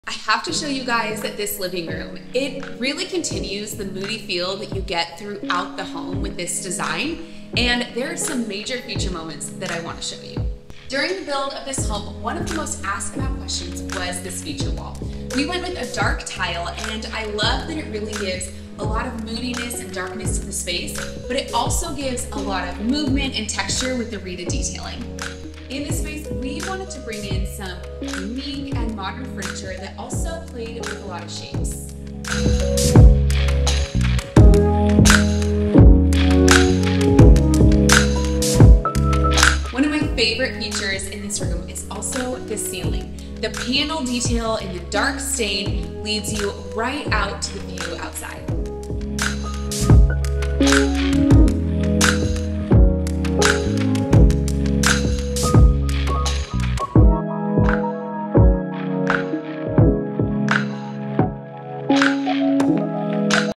CA Modern Marina - Living Room Tour
From a reeded tiled feature wall to eclectic furniture to a paneled ceiling that leads you out to the view - this moody living room continues with the over-all theme for the home. I love the texture and the vibe of this room. Perfect for watching a movie or entertaining friends.